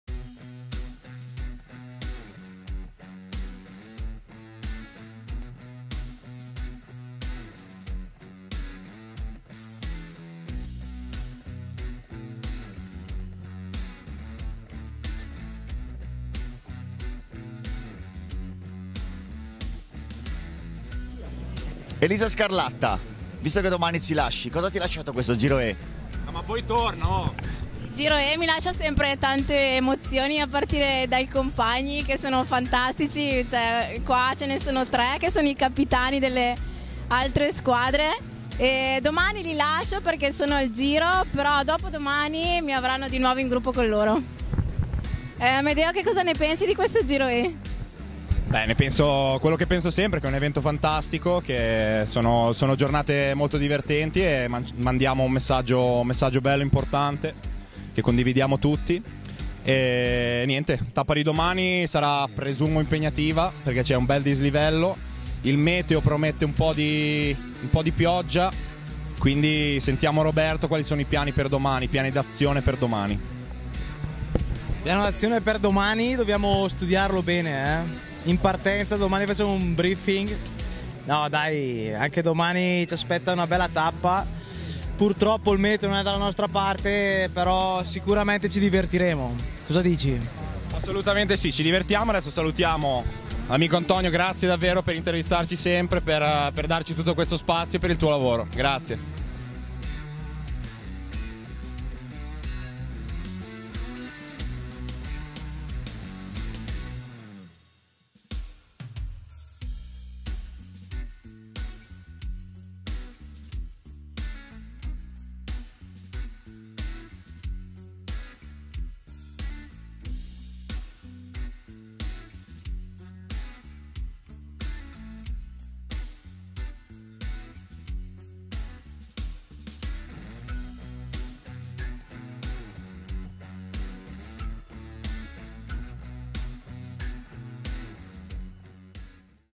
Giro E 2021 - Intervista a Moreno Moser, Elisa Scarlatta, Amedeo Tabini e Roberto Ferrari
Giro E 2021 -  Intervista a Moreno Moser, Elisa Scarlatta, Amedeo Tabini e Roberto Ferrari - 10 maggio 2021-  A cura di Antonio Abbruzzese, Giuseppe Girolamo, Filippo Mazzullo, Riccardo Cristiano, Gianni Colacione.